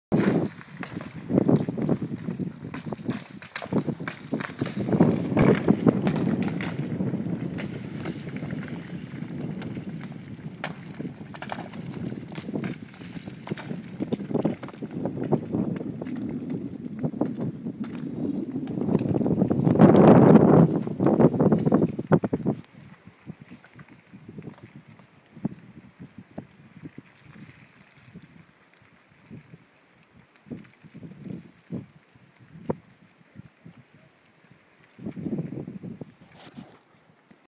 Rolling stones near Randa
On the suspension bridge near Randa, Switzerland.
The bridge opened today and makes the passage of the glacier moraine much safer and more pleasant.